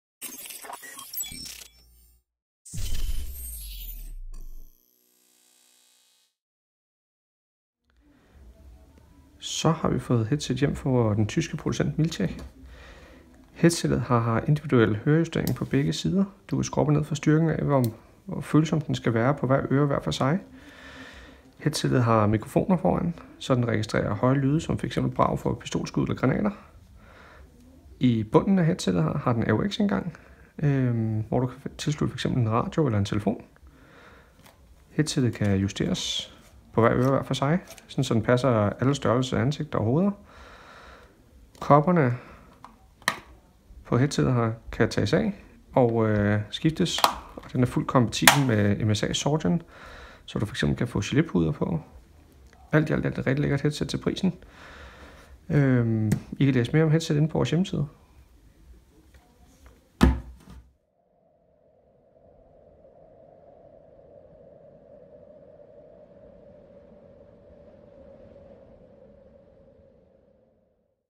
Mil Tec Aktive Høreværn
Fremvisning af de nye Mil-Tec aktive høreværn. Se mere om de spændende høreværn på vores hjemmeside her